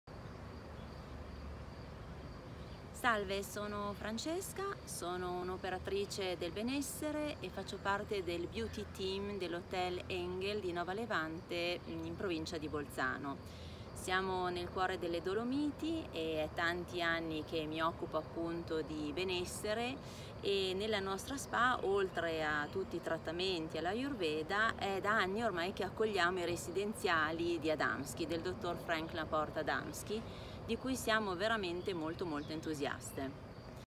Vacanza e benessere con il metodo Adamski per ritrovare una pancia piatta e migliorare il transito !
Per una pancia piatta e migliorare il transito intestinale . Aiuto al dimagrimento. Benessere e wellness senza dieta. Nuova igiene di vita e stile di vita per rallentare l’invecchiamento. Consigli alimentari e manualità sull’ addome. Workshop di 3-7 giorni per ritrovare un equilibrio mentale fisico ed emozionale.

For a flat stomach and improve intestinal transit. Slimming aid. Wellness and wellness without diet. New hygiene and lifestyle to slow down aging. Dietary advice and manual skills on the abdomen. 3-7 day workshop to regain physical and emotional mental balance.

平らな胃のためにそして腸の通過を改善しなさい。スリミングエイド。ウェルネスとダイエットなしのウェルネス。老化を遅らせるための新しい衛生とライフスタイル。腹部の食事療法のアドバイスと手動のスキル。肉体的および感情的な精神的バランスを取り戻すための3〜7日間のワークショップ。